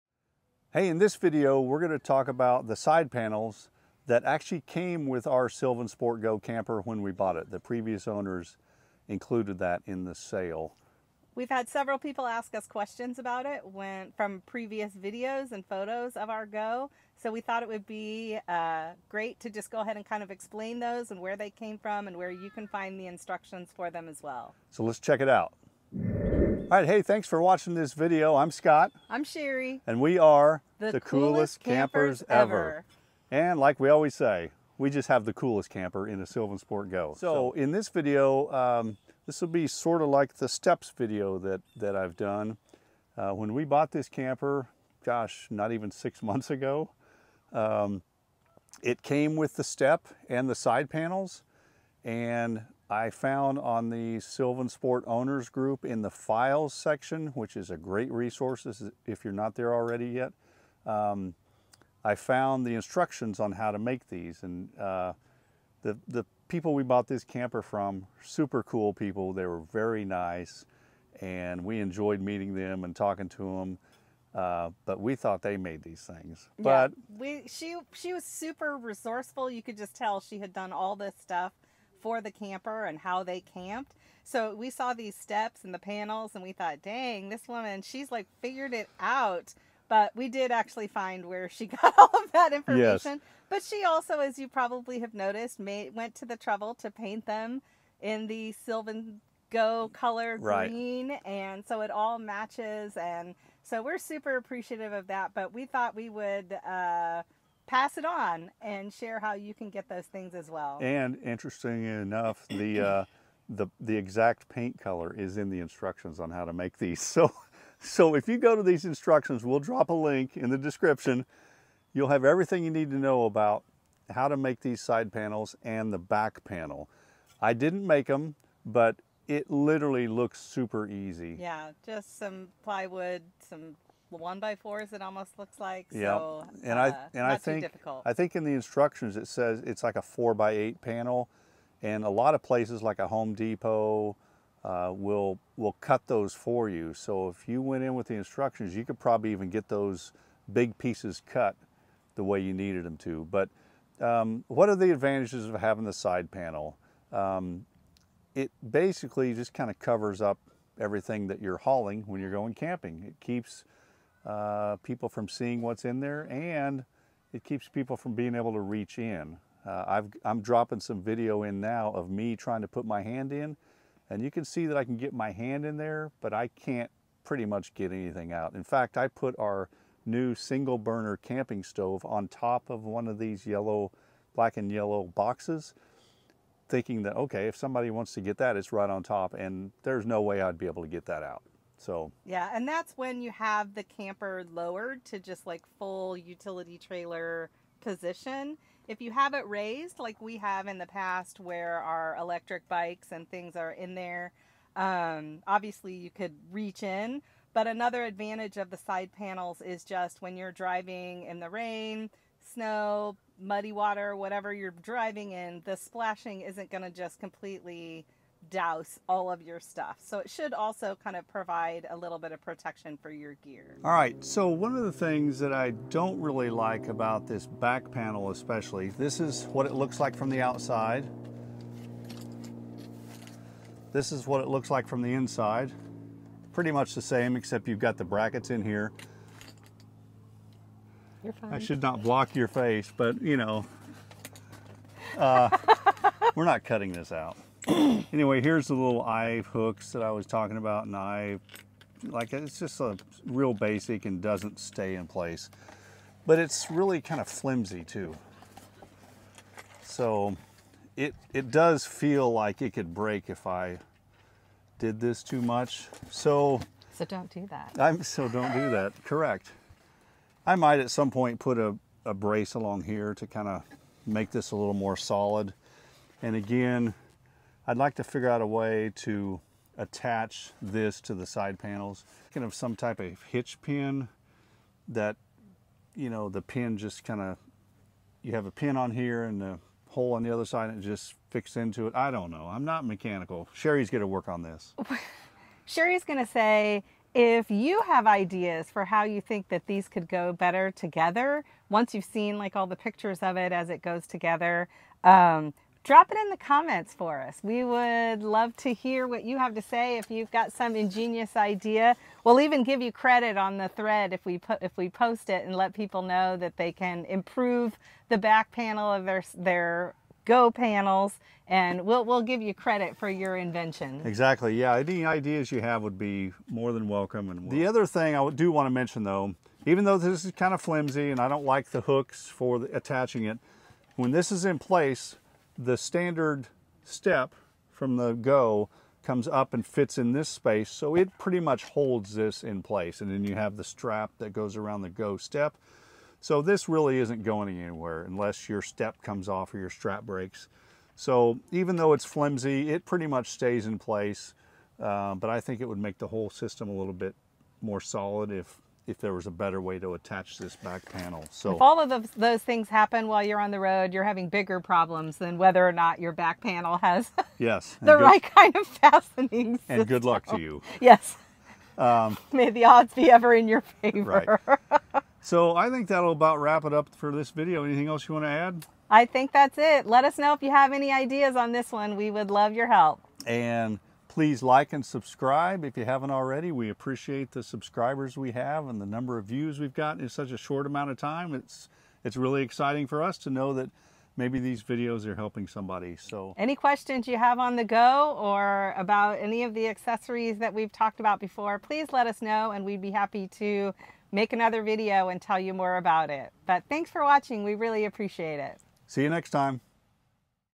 SylvanSport GO! Side Panels
This video explains a bit about the side panels may owners of the @SylvanSport GO! camper make and use, and why. We like these side panels because they make it a little more difficult to see what's inside the trailer when traveling and they can keep water and mud from splashing on items in the trailer.

SPECIAL THANKS goes to Dan Schallenkamp who originally shared this idea/design on the SylvanSport Owners Group on Facebook. If you're not following this page, you should be! It has the friendliest people in the group and the "Files" section includes plans like the ones needed to make these panels.

Here is a link to the PDF instructions about how to make these side and end panels